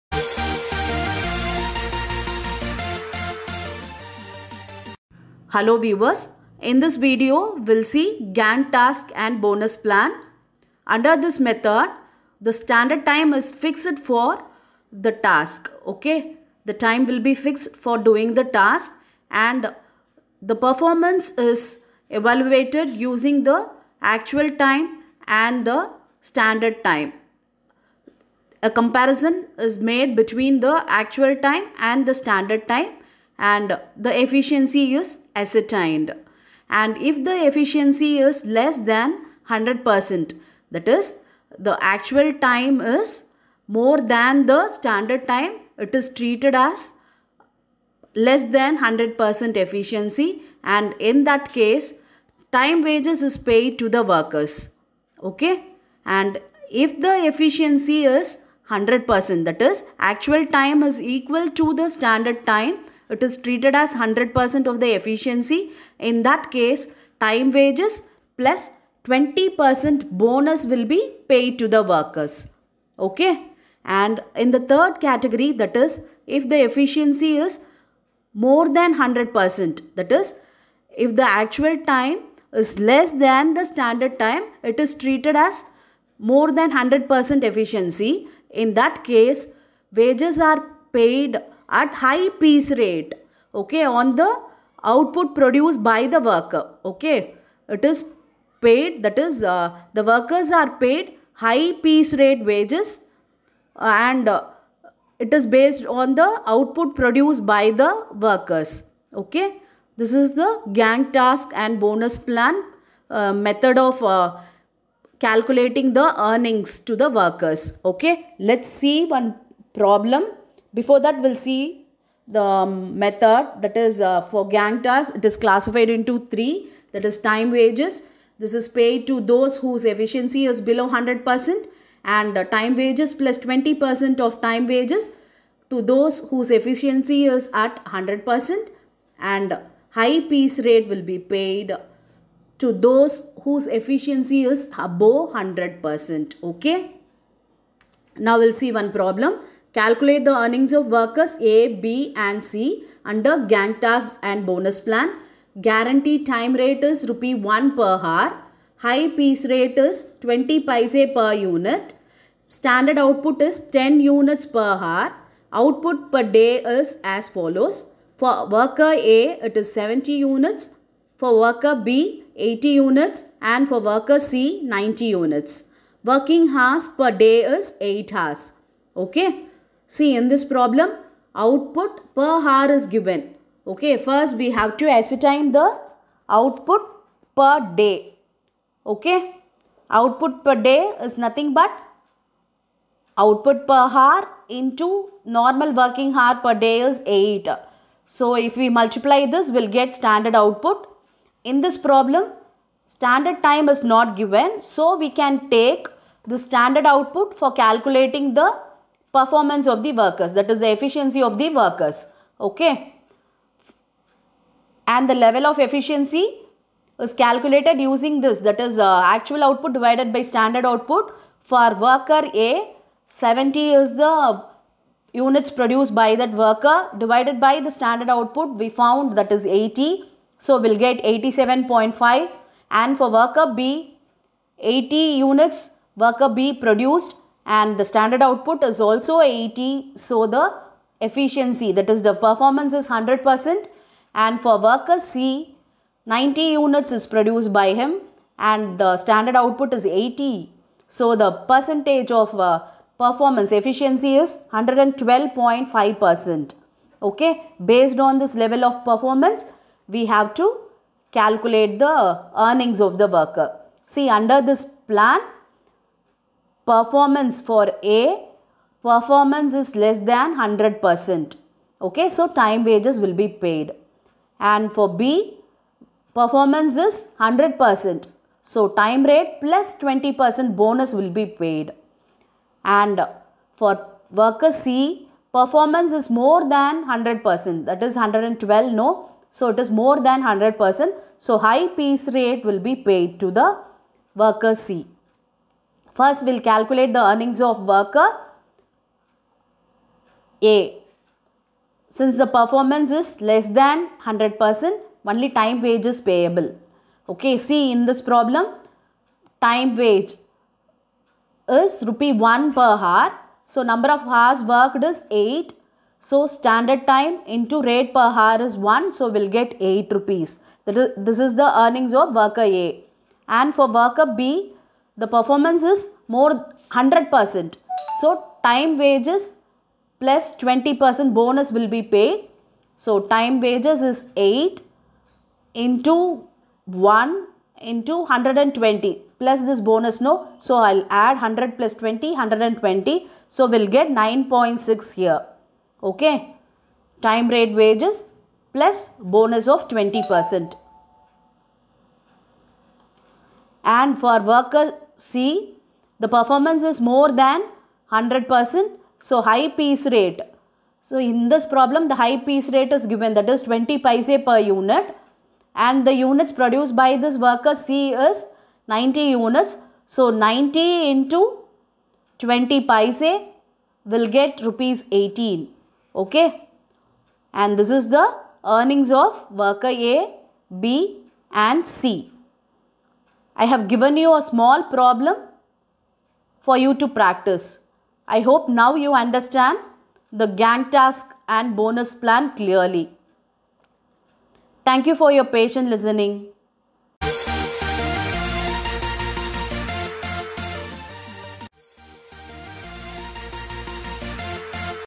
Gantt's Task and Bonus plan
Problem with solution under Gantt's Task and Bonus plan